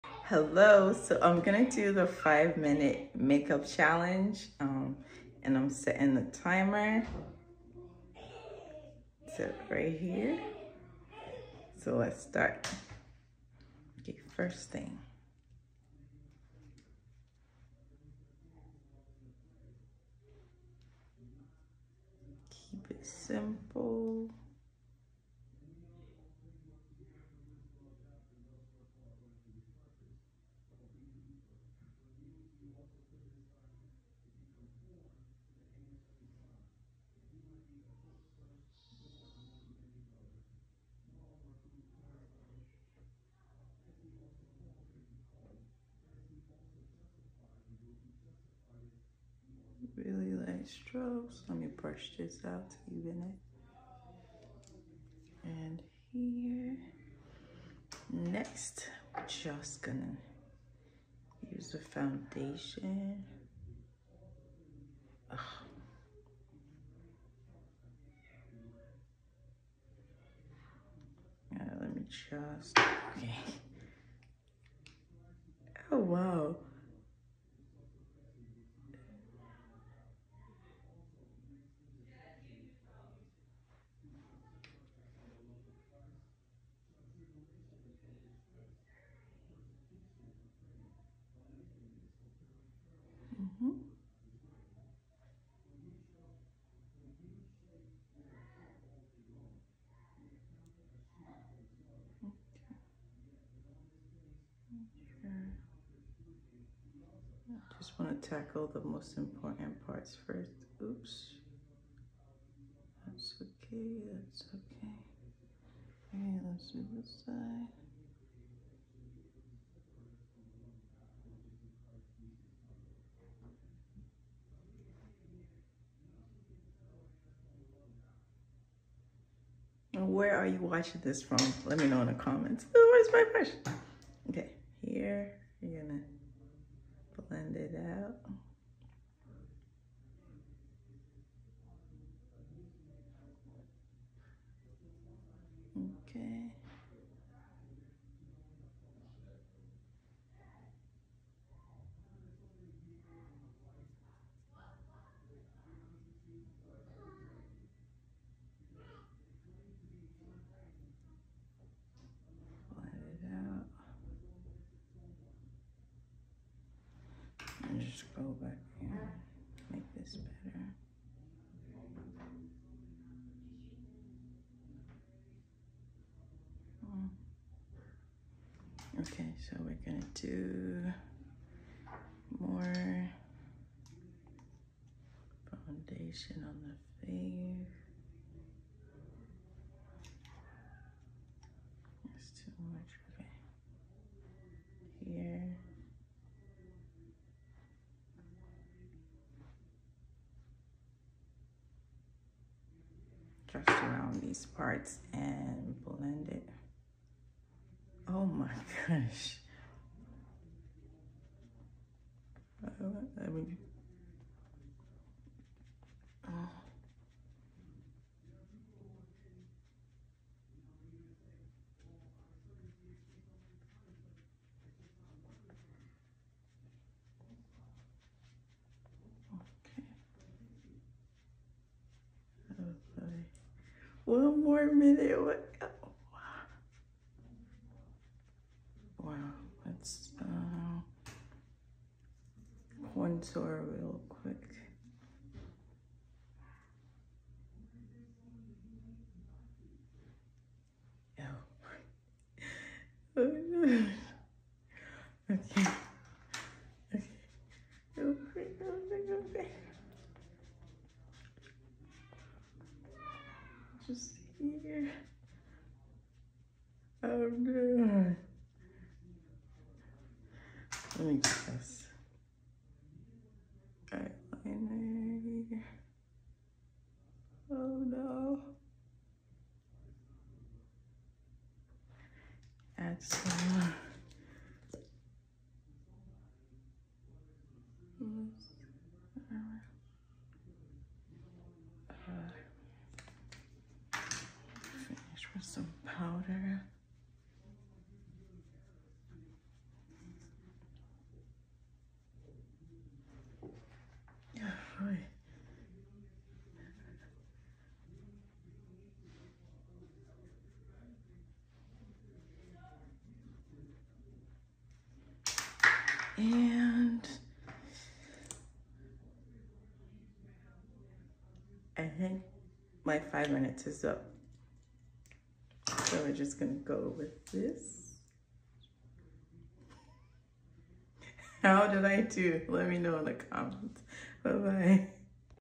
MOM OF 7 TAKES ON THE 5 MINUTE MAKEUP CHALLENGE
Have you ever had to run out the house so quick you barely had the time to look nice.what if you only had 5 minutes to apply makeup before dashing out the house?

I took on the perfect challenge to push myself and see what makeup look I can come up with in 5 minutes and let me tell you those minutes go by so quick! 😂

Makeup challenge, mom life, viral, makeup look, beauty, makeup